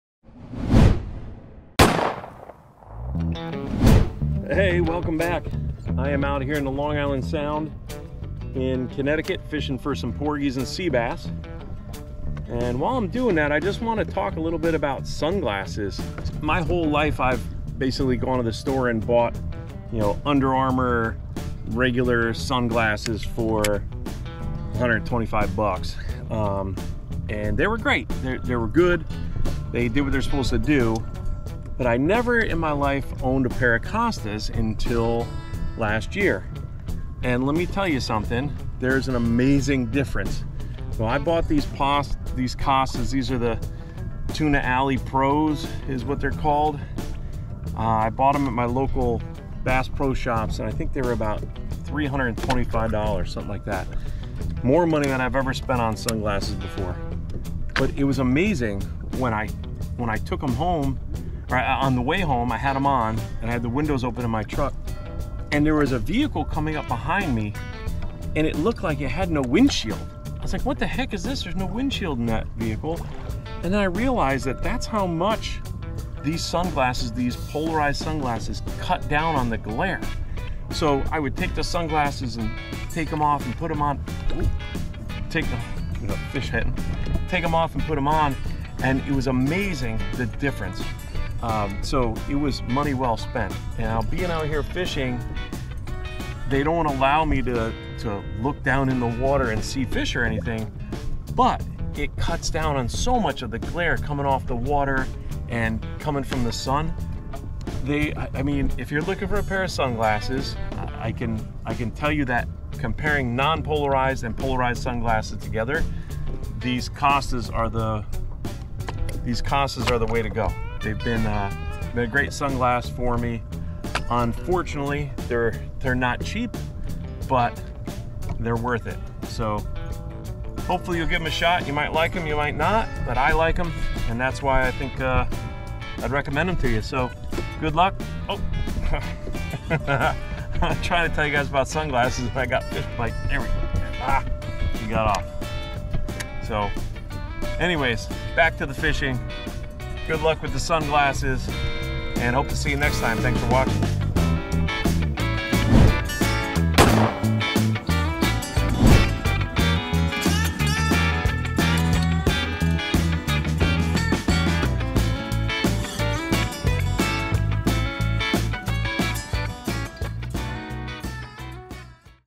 Costa Tuna Alley Pro Sunglasses
I love these glasses so much, I figured I would make a quick video about them.  - Tight Lines 'n Short Blood Trails
Here's a link to buy these sunglasses if you like them